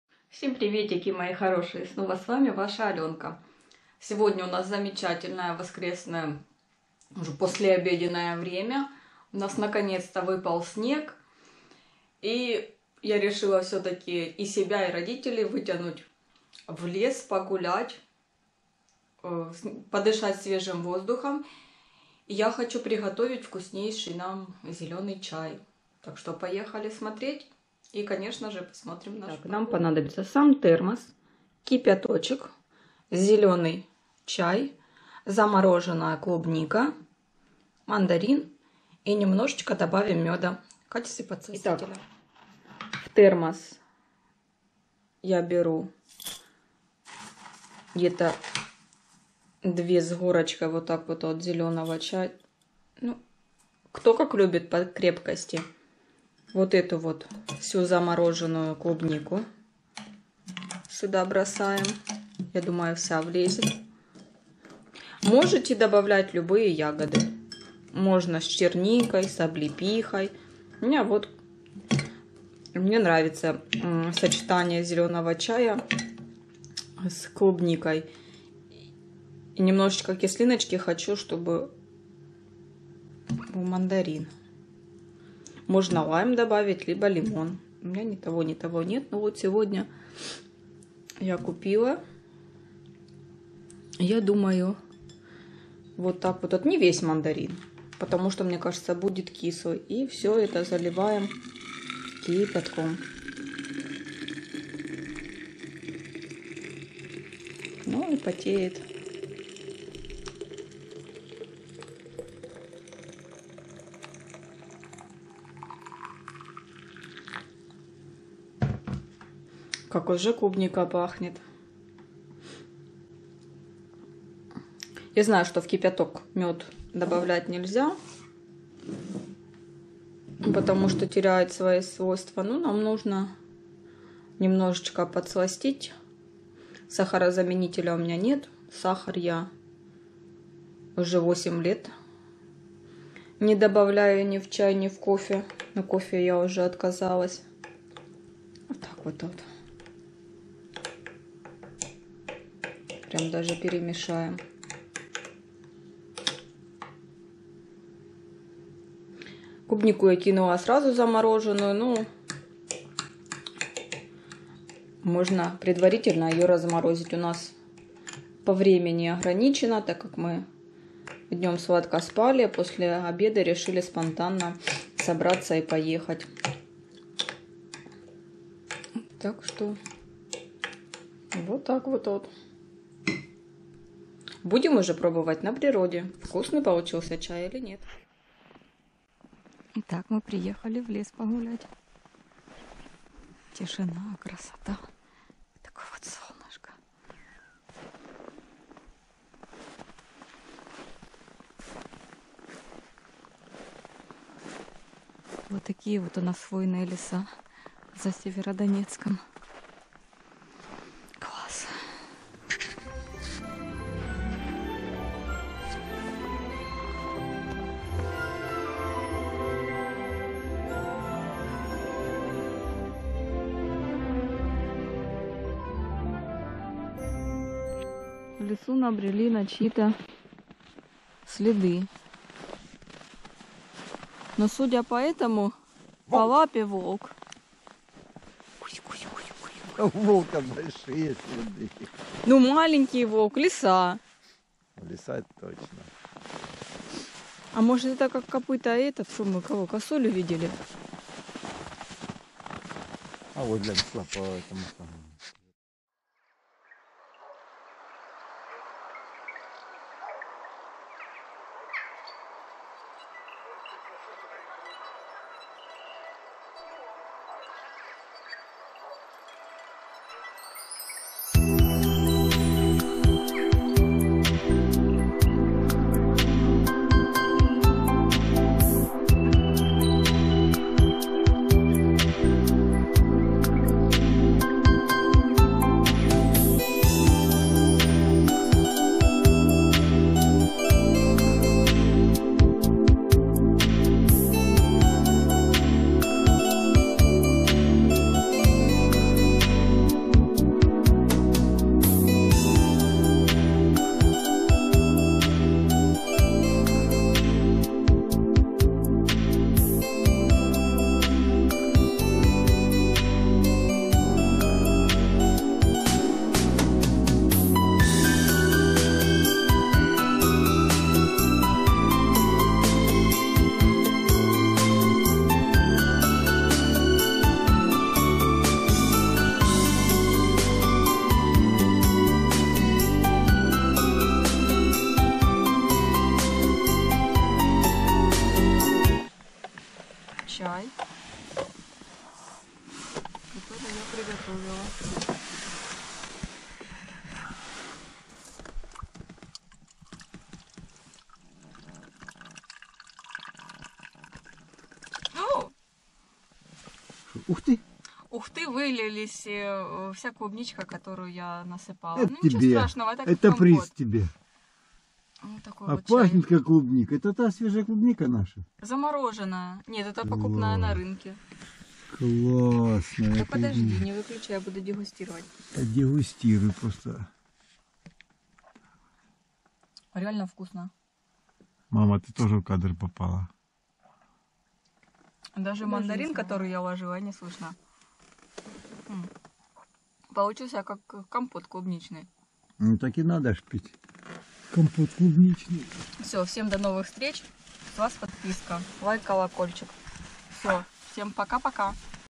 РЕЦЕПТ: Вкусный зеленый чай | Прогулка по зимнему лесу в Северодонецке
Воскресная прогулка по зимнему лесу. Пьем вкуснейший зеленый чай с клубникой и мандарином.